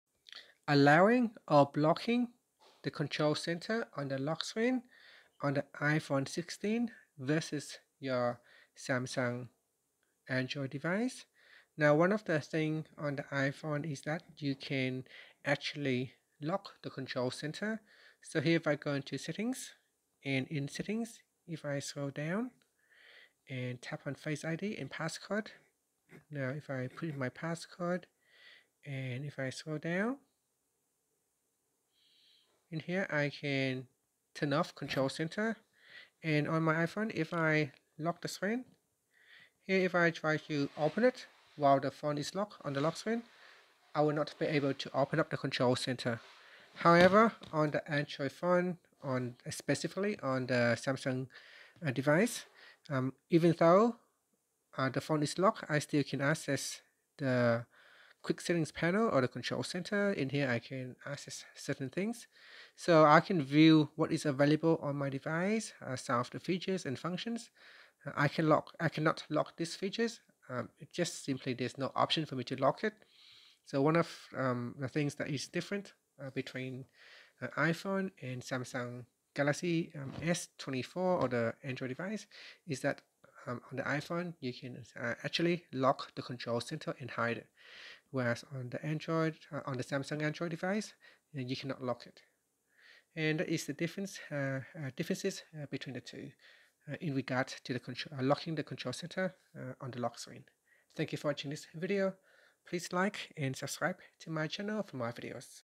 📲 Control Center Access on Lock Screen: iPhone vs Samsung | Security Guide 🔐
Learn how to manage Control Center access on the lock screen for both iPhone and Samsung Android phones! This ITJungles guide explains how to allow or block Control Center access, comparing security settings on iOS and Android. Find out which options can enhance your phone’s privacy and security, preventing unauthorized access while your device is locked. Perfect for anyone looking to secure their device or understand the differences between iPhone and Samsung lock screen features.